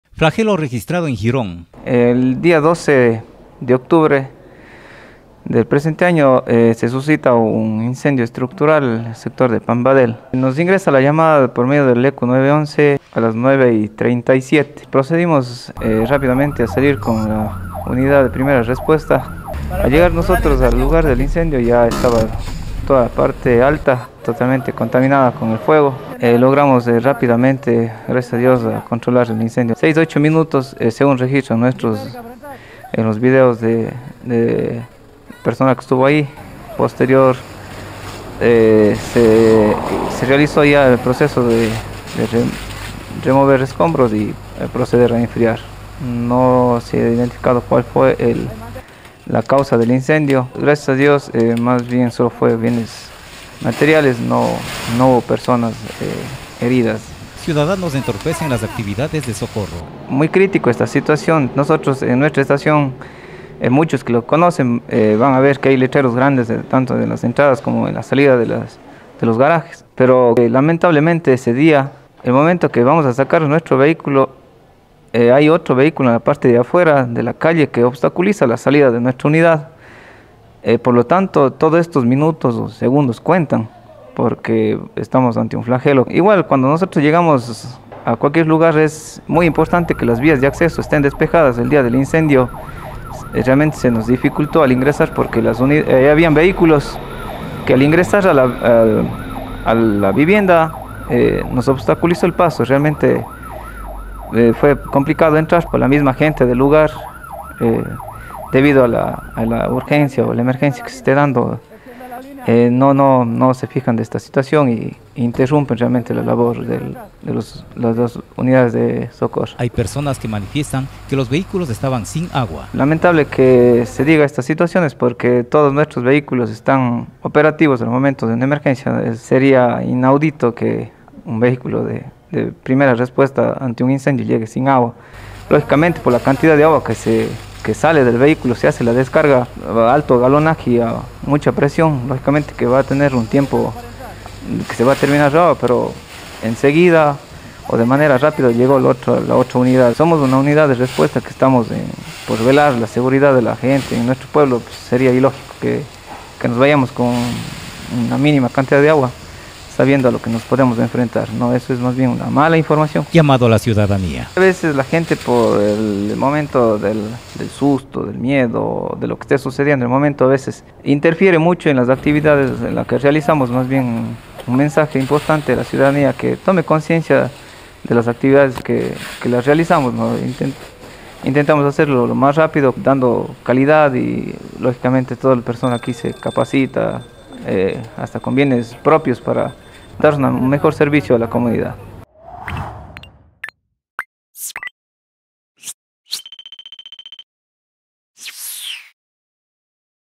Incendio de una vivienda en San Juan de Pambadel, los bomberos lo registraron en video
El martes 12 de octubre, los bomberos gironenses asistieron a un incendio estructural en Pambadel, donde una vivienda por razones aún desconocidas fue consumida por las llamas, no hubo personas afectadas por el flagelo pero sí grandes daños materiales. Los socorristas, tienen registrado su accionar en videos captados desde el casco del primer jefe, con lo cual se desvirtúan comentarios mal intencionados de que se asiste sin agua; además dicen que la actitud de muchos ciudadanos y conductores entorpecen las actividades de socorro.